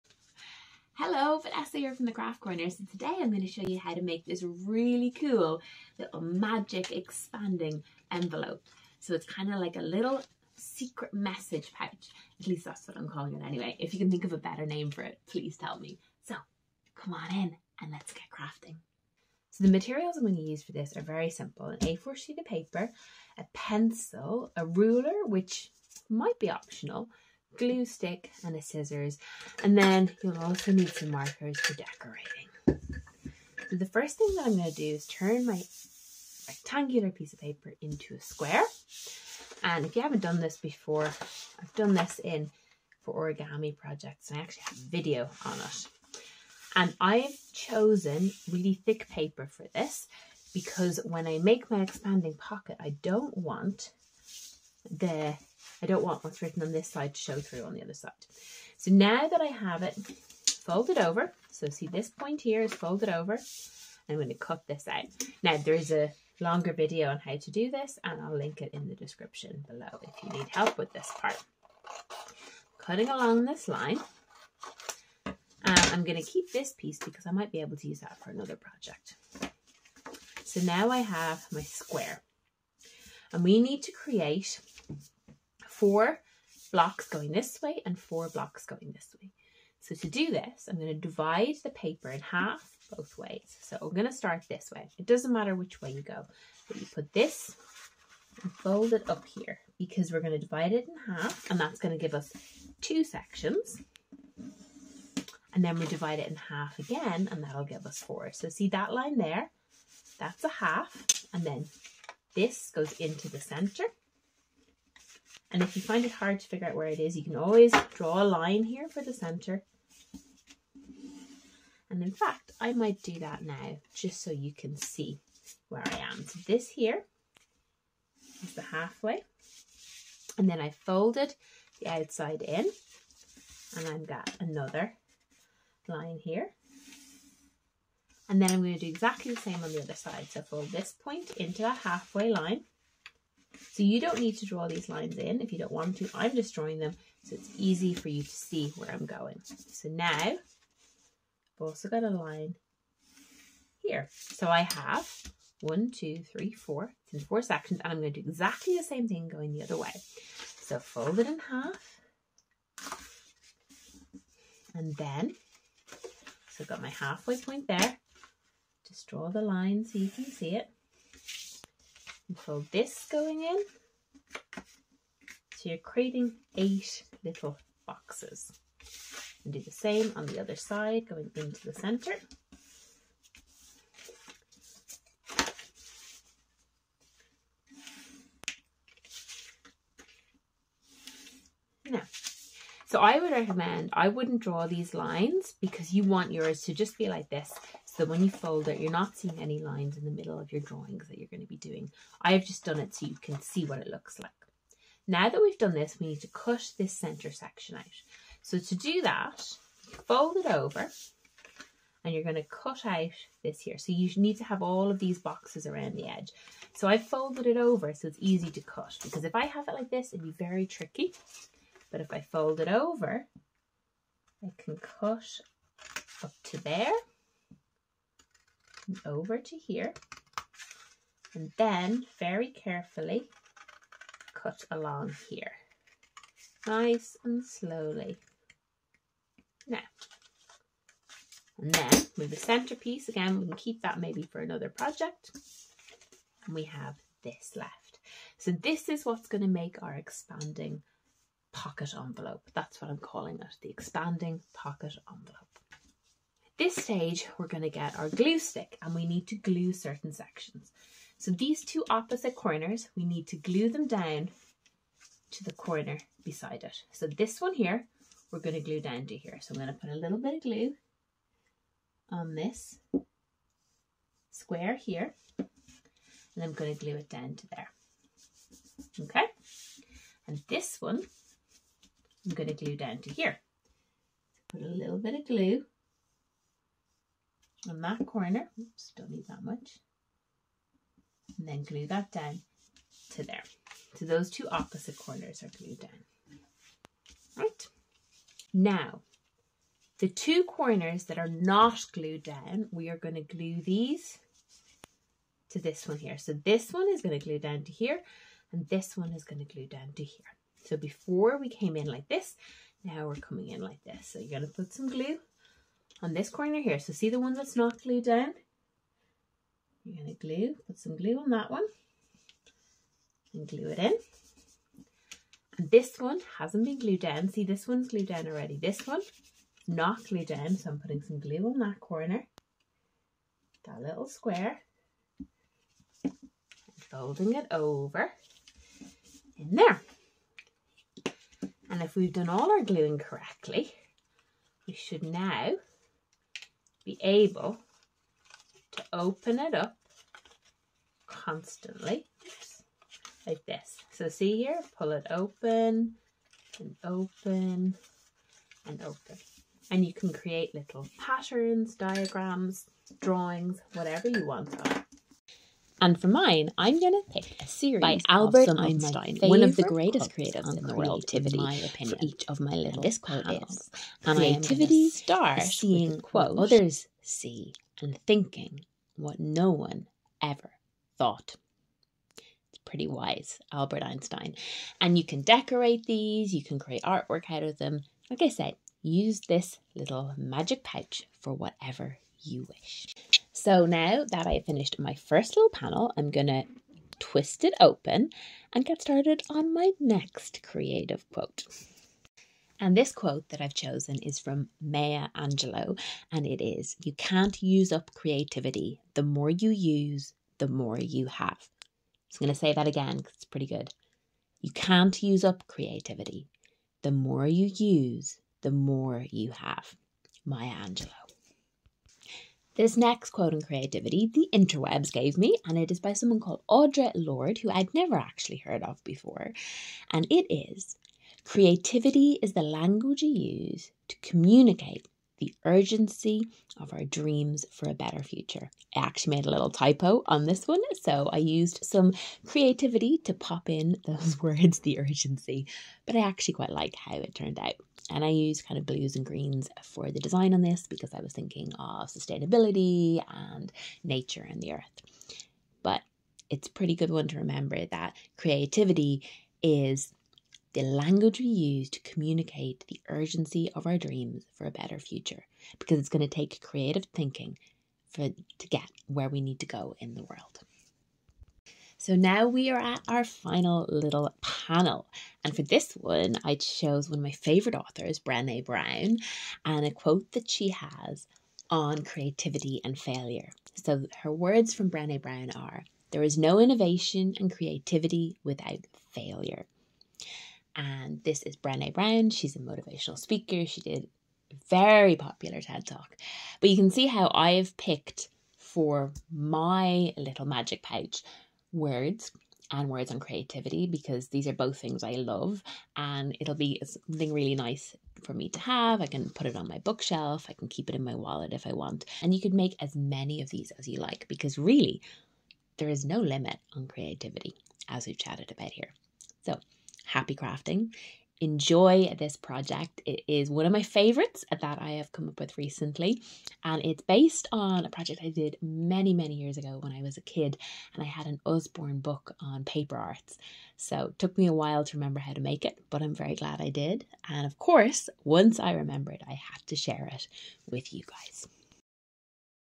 Making magic note pockets with the Craft Corner. paper folding projects for children.
Learn to make this simple one page magic note pocket with me, Vanessa from The Craft Corner.
Yes, I'm still not entirely sure what I should call them, I recreated this project from one that I remember making when I was a child (many, many moons ago) that I think may have been in an Usborne paper crafts book.

Materials are very simple.
One sheet of thick paper
Scissors
Pencil
Glue stick
Ruler might be handy
Colours

I filled my magic pocket with lots of my favourite quotes on creativity from Albert Einstein to Maya Angelou and Brene Brown

But you could write secret love notes, letters, artworks. Create miniature story books.

Really the possibilities for this project are endless. And those are my favourite kind, as once you know the method you can just keep on making, and making and making some more!